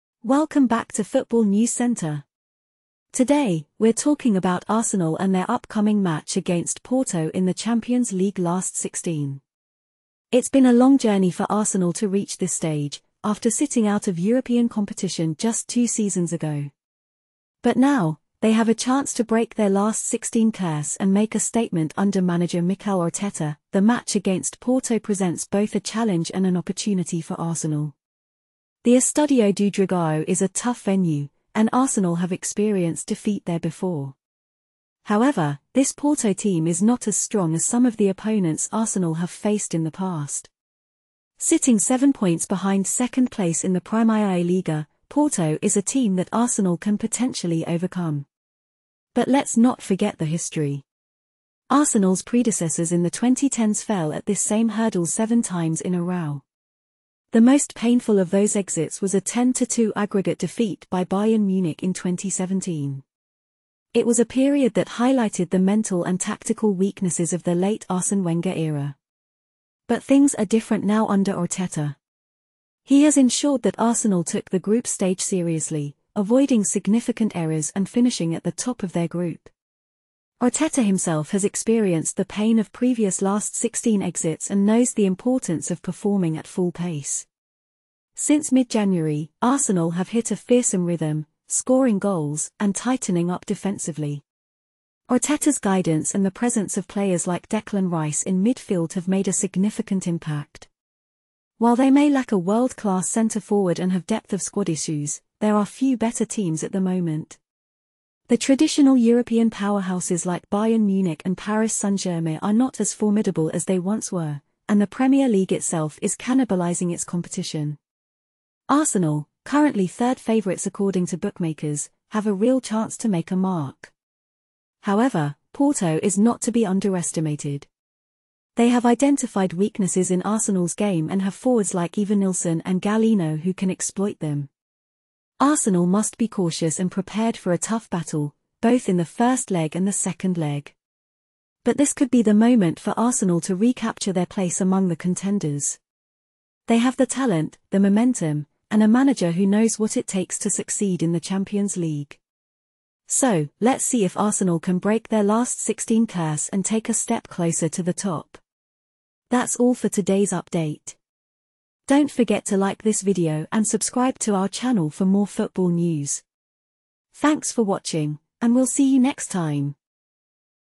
Arsenal face Porto looking to break last-16 curse of their predecessors | Nick Ames
Arsenal is set to face Porto in the Champions League last 16, and breaking their curse of not progressing beyond this stage since 2010 is a priority for the club. The draw has given them an opportunity to cast off historical baggage and face a relatively weak field of competitors. Mikel Arteta, Arsenal's manager, is determined to avoid the mistakes made by his predecessors in the 2010s, who fell at this hurdle in seven consecutive campaigns. Arsenal's mental and tactical weaknesses were exposed during those years, but Arteta has instilled a different mindset and approach in the team. Since mid-January, Arsenal has been in a strong rhythm, scoring goals and conceding few shots. While they may lack a standout center-forward and have depth-of-squad issues, they are still considered one of the better teams in the competition. The Premier League's dominance and the uncertainty surrounding other traditional European powerhouses have created an opportunity for Arsenal. However, they must be cautious and prepared for the challenge posed by Porto, who have identified weaknesses in Arsenal's defense. Arsenal is ready to recapture their place among the contenders and progress in the tournament.